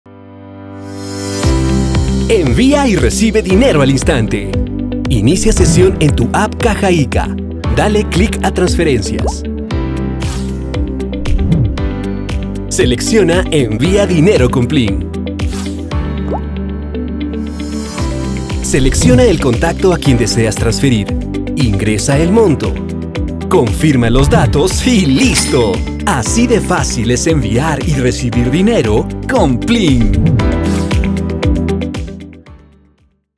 Paga rápido con PLIN 😎
¡Envia y recibe dinero en un PLIN!😎
Te enseñamos como transferir dinero en un instante📱💸
Recuerda descargar PLIN desde tu app de Caja Ica